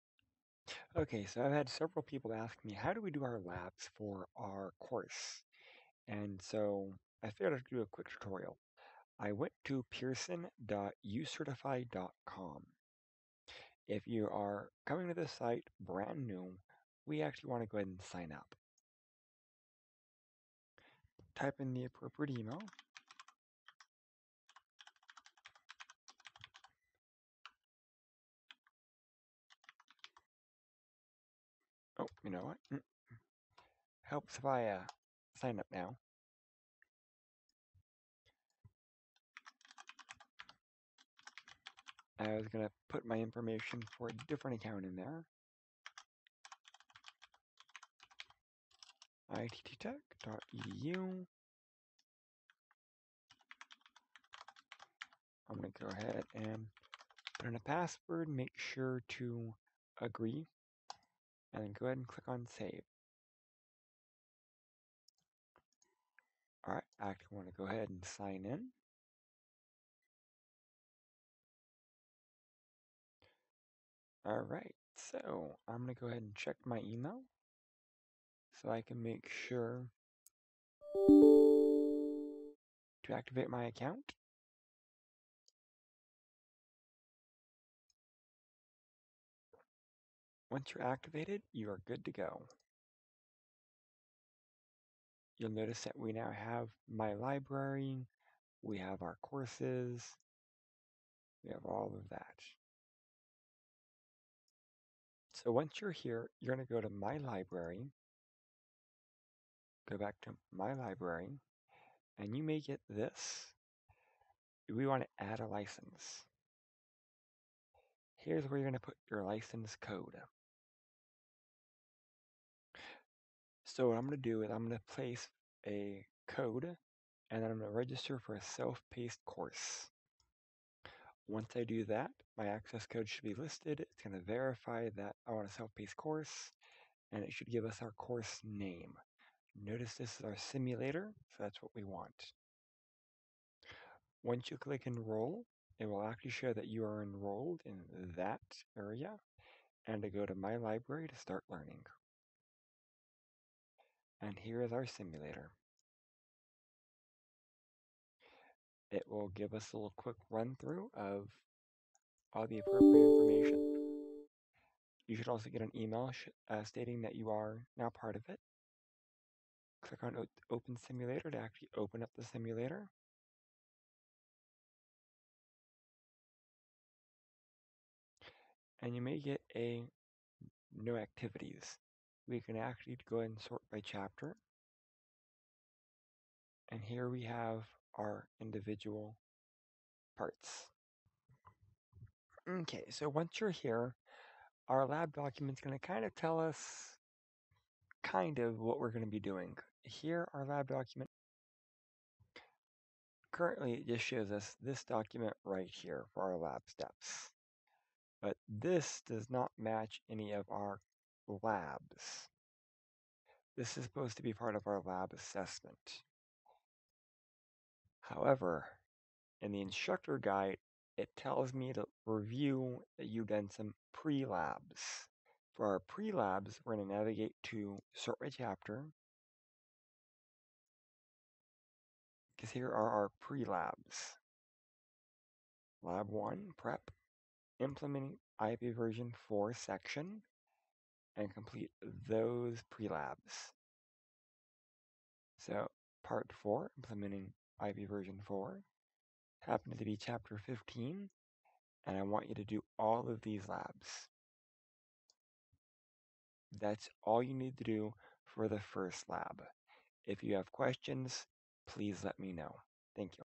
NT 2540 - Lab Prep
How to get your account setup and start your first labs.